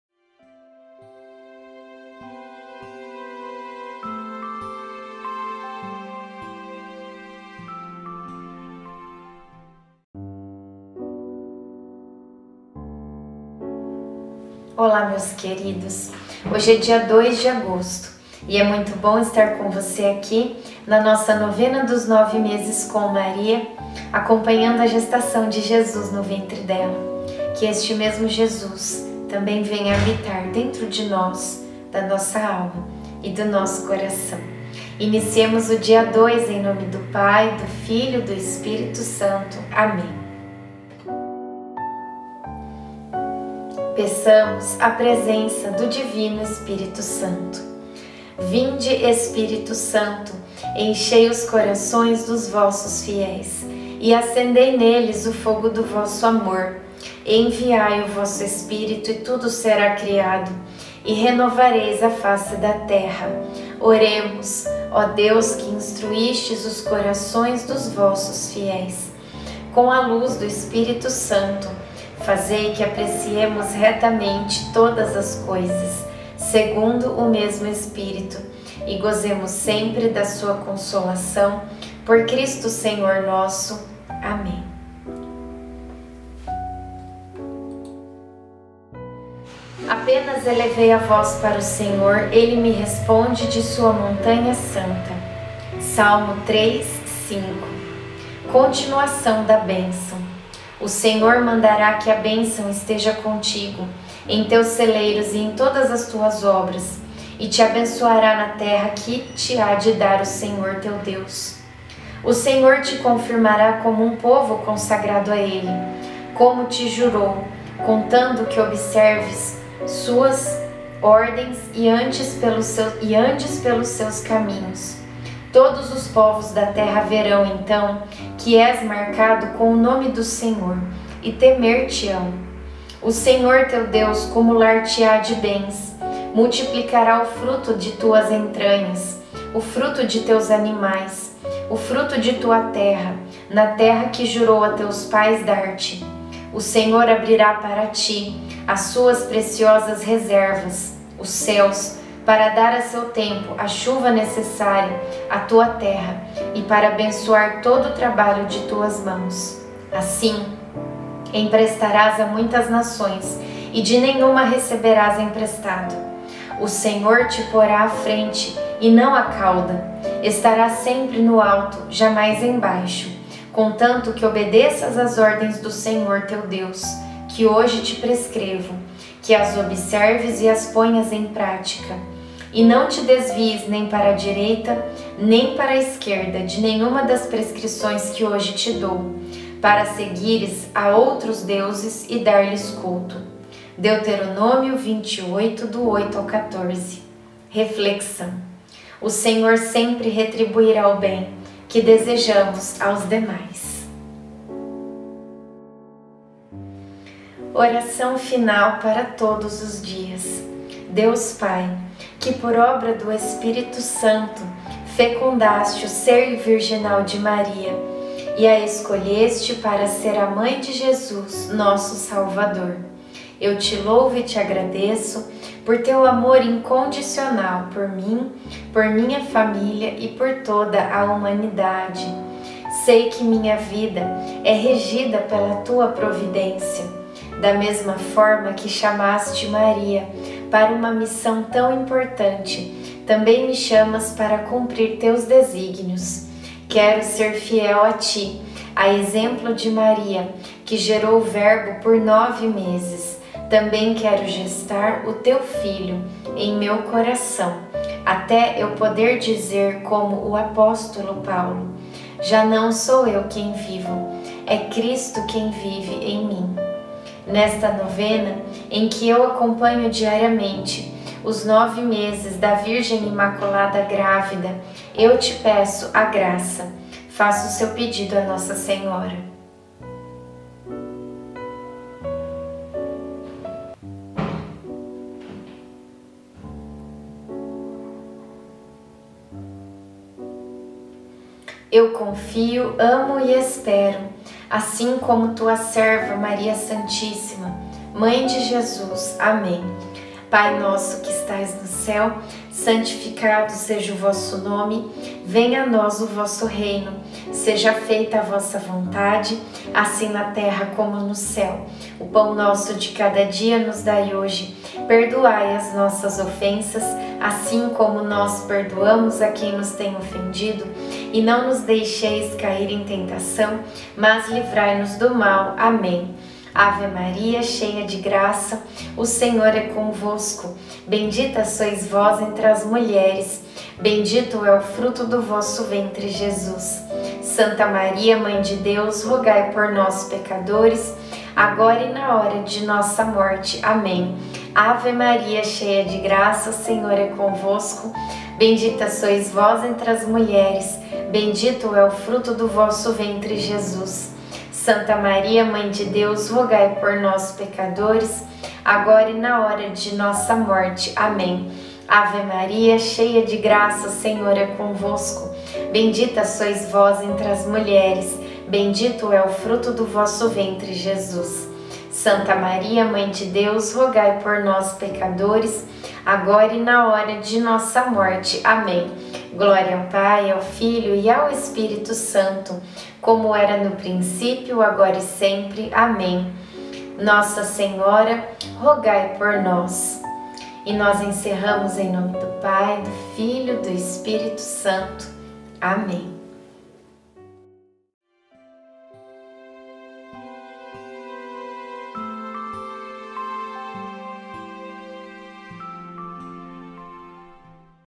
NOVENA 9 MESES COM MARIA - 02/08
2 de agosto

Apenas elevei a voz para o Senhor, ele me responde de sua montanha santa. (Salmo

3,5)

Continuação da bênção: O Senhor mandará que a bênção esteja contigo, em
teus celeiros e em todas as tuas obras, e te abençoará na terra que te há de dar o
Senhor, teu Deus. O Senhor te confirmará como um povo consagrado a ele, como
te jurou, contanto que observes suas ordens e andes pelos seus caminhos. Todos os
povos da terra verão, então, que és marcado com o nome do Senhor, e
temer­te­ão. O Senhor, teu Deus, cumular­te­á de bens, multiplicará o fruto de
tuas entranhas, o fruto de teus animais, o fruto de tua terra, na terra que jurou a
teus pais dar­te. O Senhor abrirá para ti as suas preciosas reservas, os céus, para
dar a seu tempo a chuva necessária à tua terra e para abençoar todo o trabalho
de tuas mãos. Assim, emprestarás a muitas nações, e de nenhuma receberás
emprestado. O Senhor te porá à frente e não na cauda; estarás sempre no alto,
jamais embaixo, contanto que obedeças às ordens do Senhor, teu Deus, que hoje te
prescrevo, que as observes e as ponhas em prática, e não te desvies nem para a
direita nem para a esquerda de nenhuma das prescrições que hoje te dou, para
seguires a outros deuses e dar­lhes culto. (Deuteronômio 28,8-14)
O Senhor sempre retribuirá o bem que desejamos aos demais.

Deus abençoe!